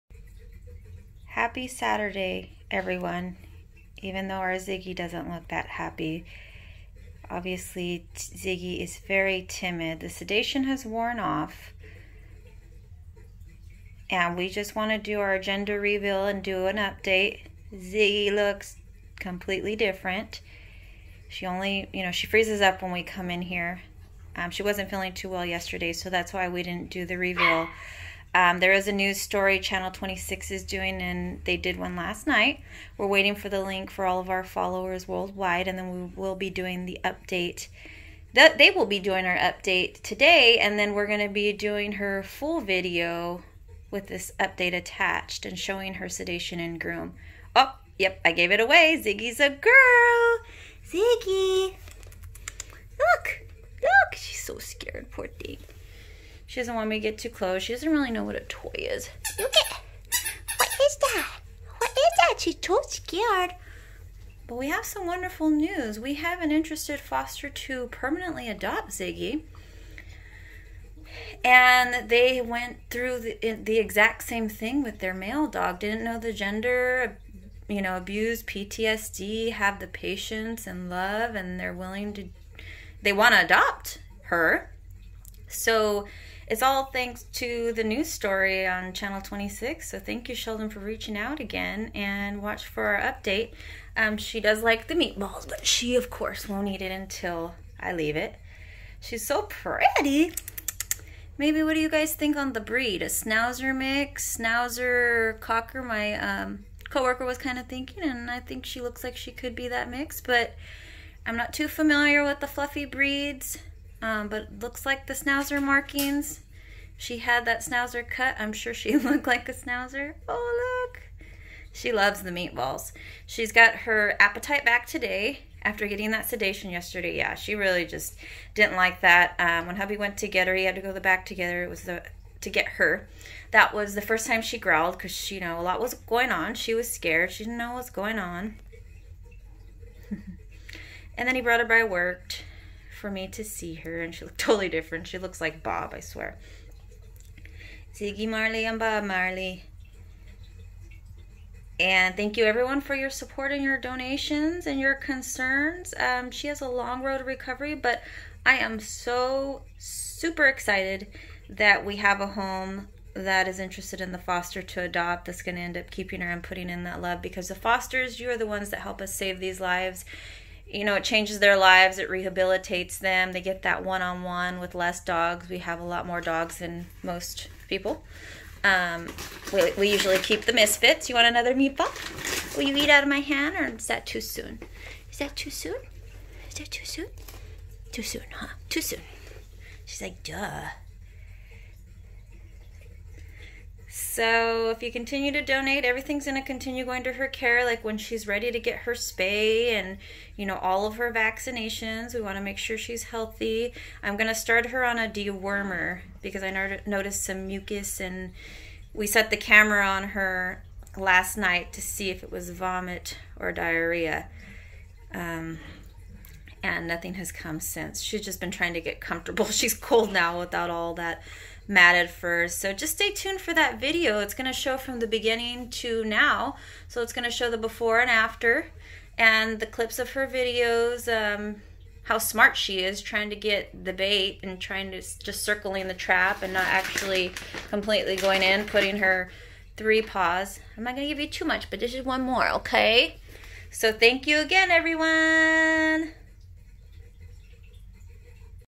Gender reveal! After sedation and shavedown.
The day after Ziggy's vet visit. She wasn't feeling too well after sedation. We give her space since she is scared of human interaction.